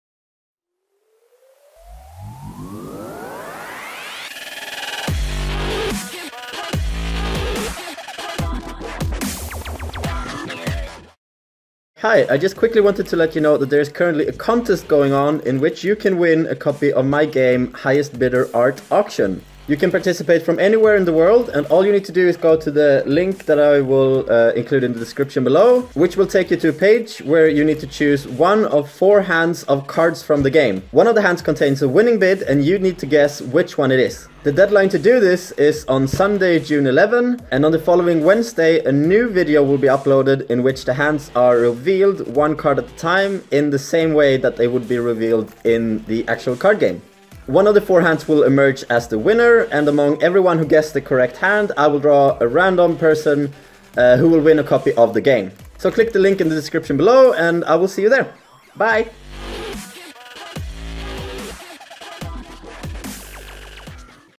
Highest Bidder Giveaway Announcent
I'm giving away a copy of my card game "Highest Bidder: Art Auction"!